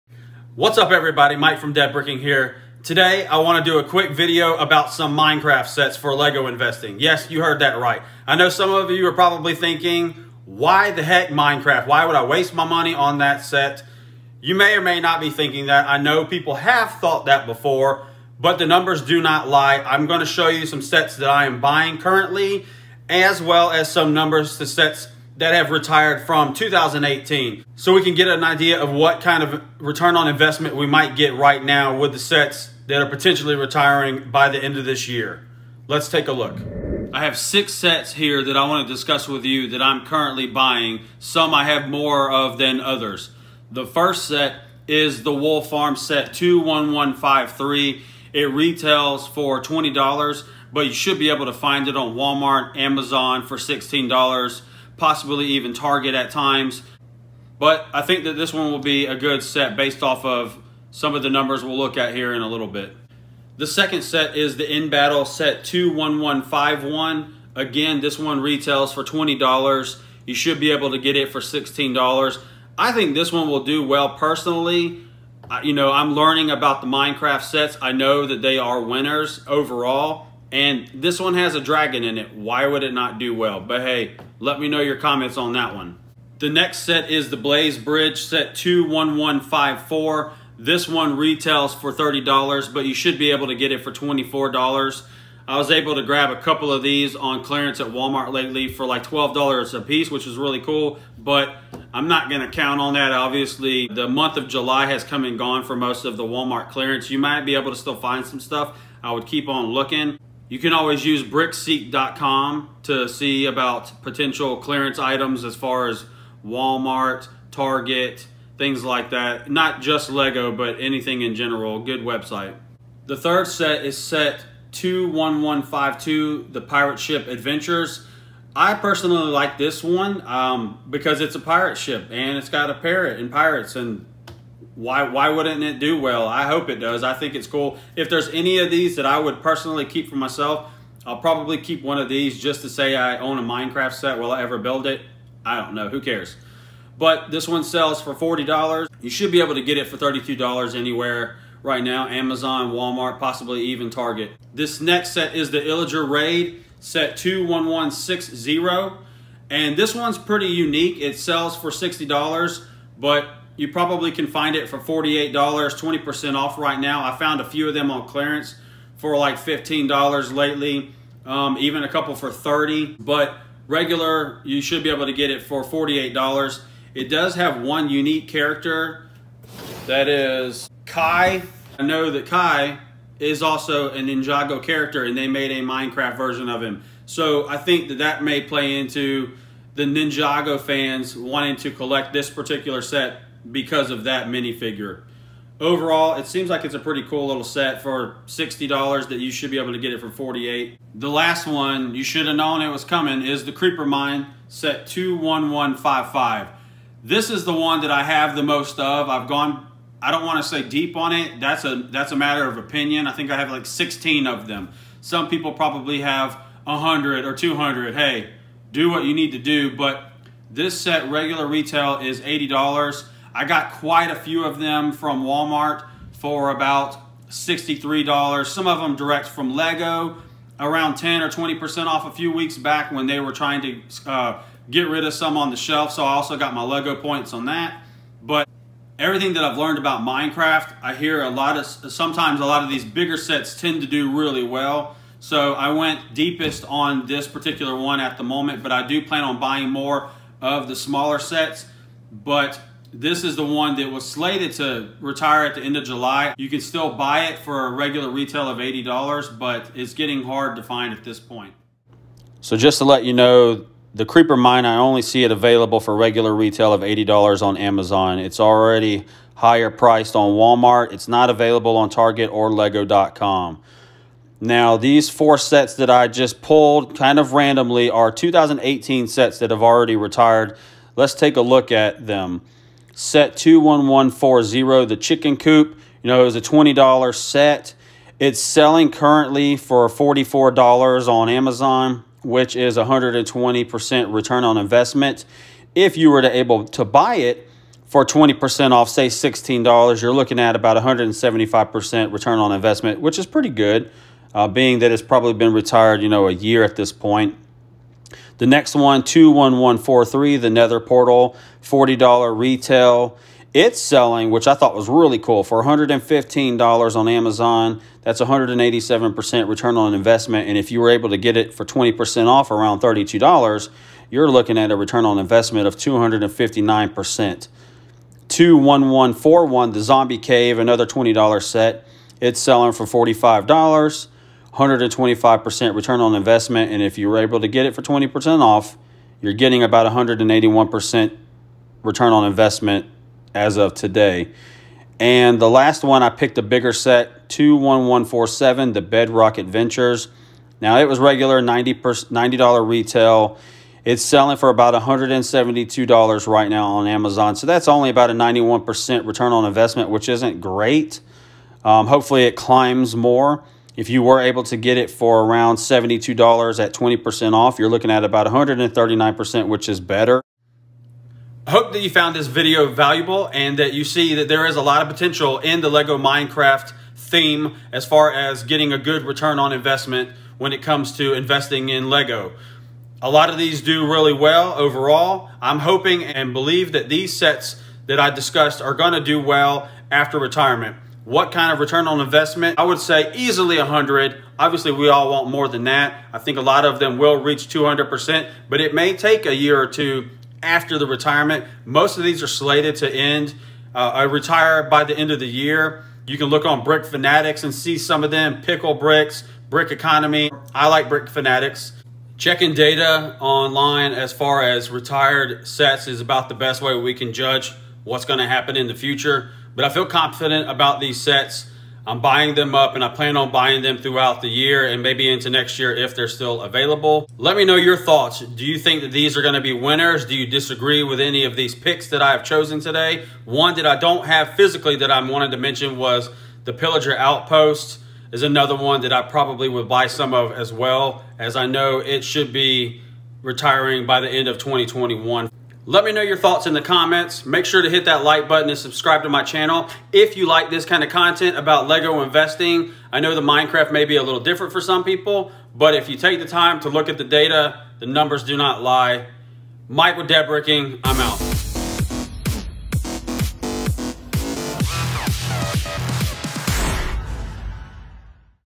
Why Invest in this LEGO?
I go over an easy LEGO to invest in that many people pass up.